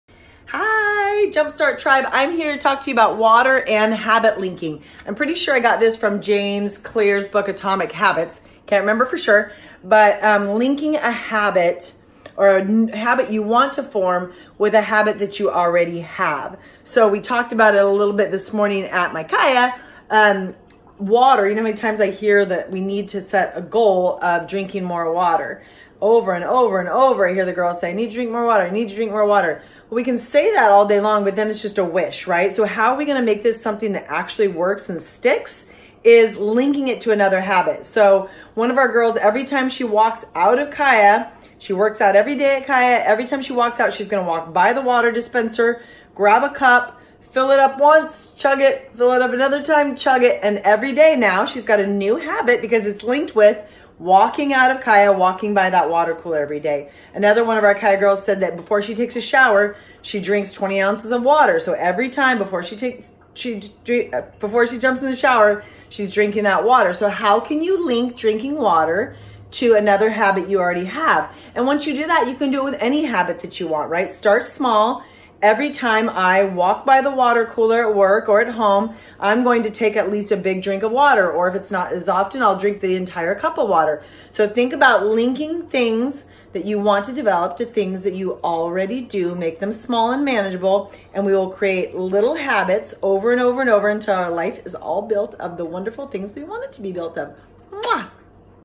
Habit Linking
Create habits that you can easily do day after day that support your goals.